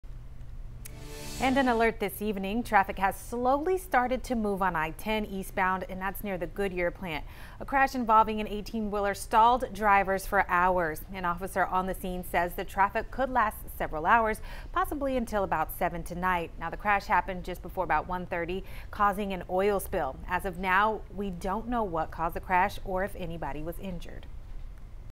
I-10 backup south of Beaumont slowly clearing after crash involving 18-wheeler
An officer on scene said the backup could last until 6 or 7 p.m. due to a chemical spill.